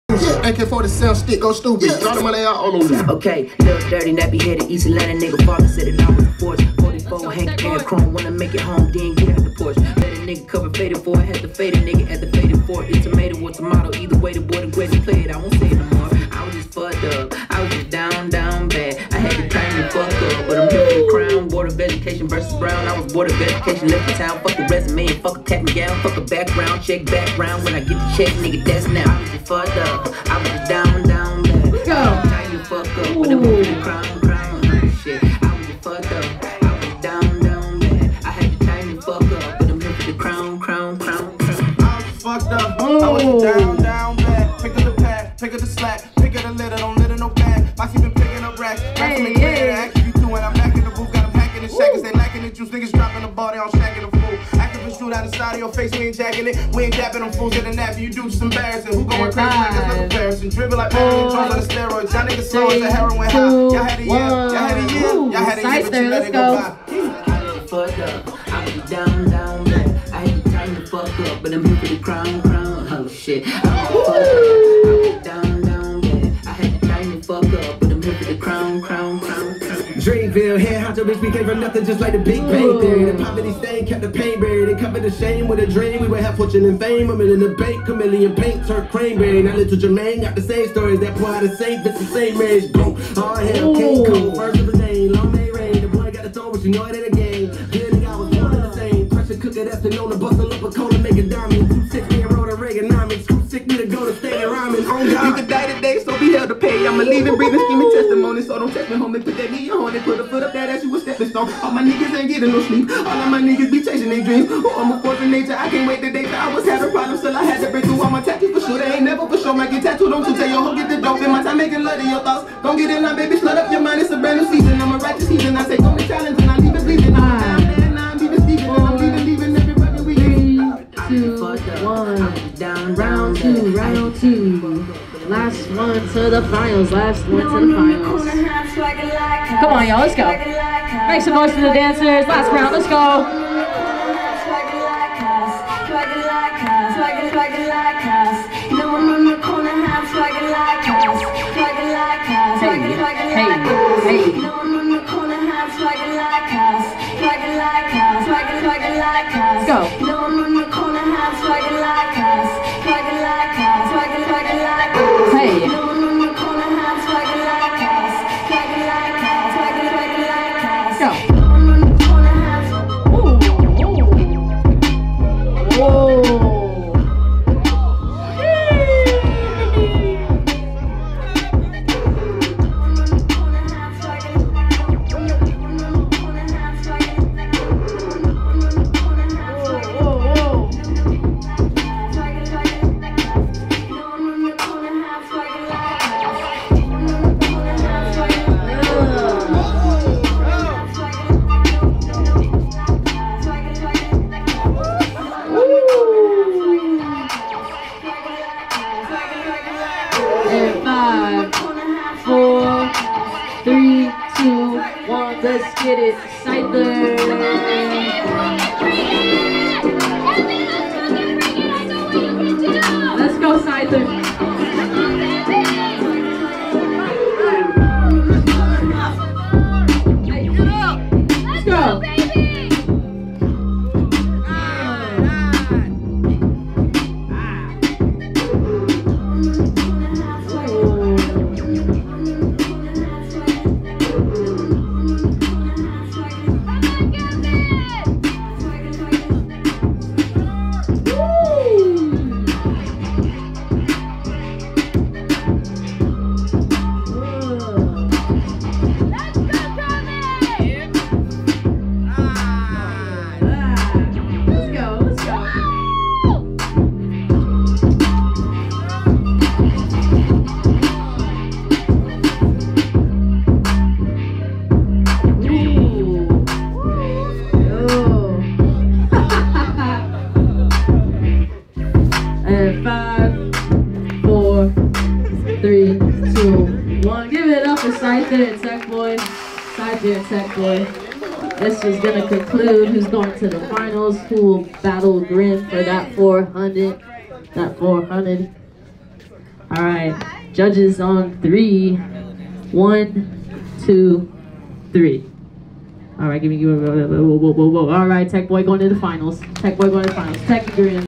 Tec vs Scyther Semi Battle At The Babylon Vol. 2 - Open Styles
.UndergroundBreakingTV

Judges:
Shinobi Jaxx
Koncept
Prevail
Shayna Vu
Integra